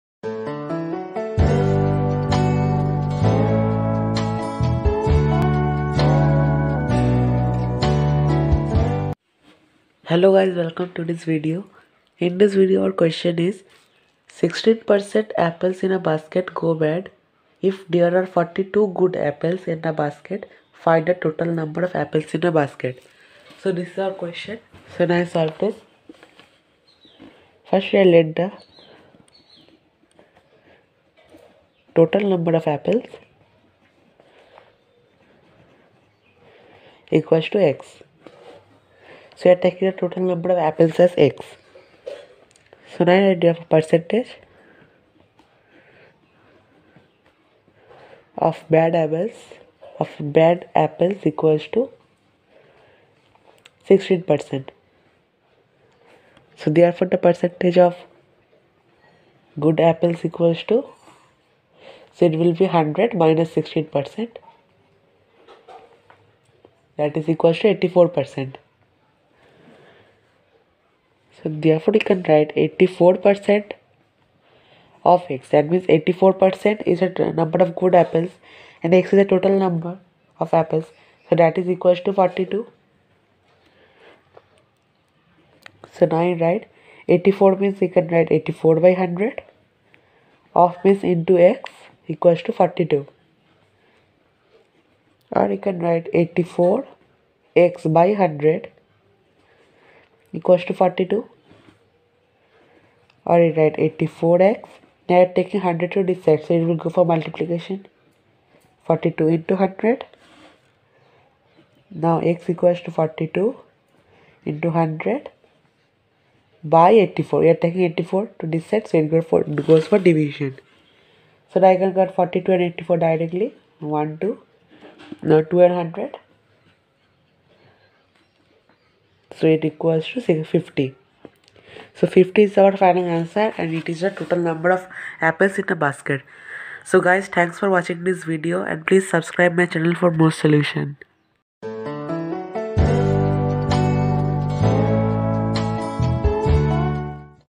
16% of the apples in a basket go bad. If there are 42 good apples in the basket, find the total.....
16% of the apples in a basket go bad. If there are 42 good apples in the basket, find the total number of apples in the basket.